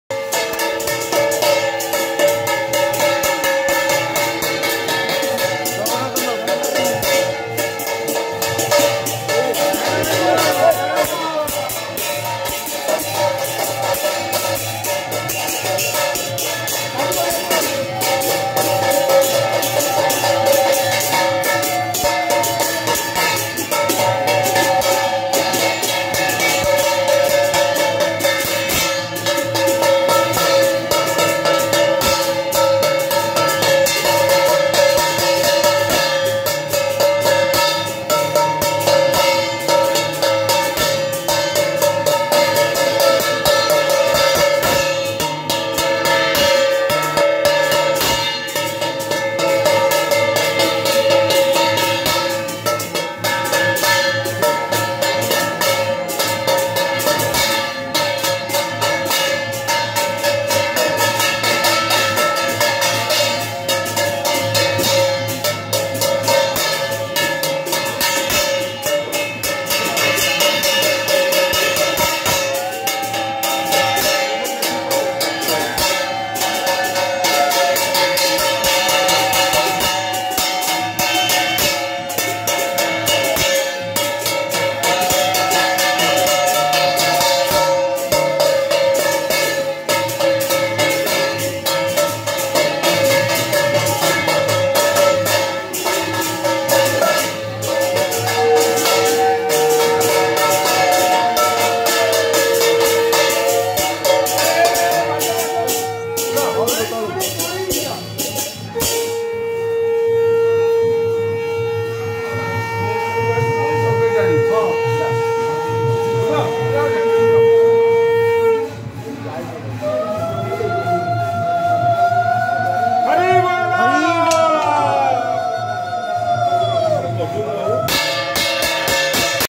Miracle of Sidha Barang Gosein Pitha and Puri Jagannath Mahaprabhu,Jagatsinghpur,Odisha
Miracle of Sidha Barang Gosein Pitha ,Jagatsinghpur due to once in a year on the occasion of Bhoj purnima lord Jagannath visit to this place and as per lord jagannath puri mahaprasad preparation process followed and when this malachula bend, the preparation process completed.

Video from Satyajit Chakrabarty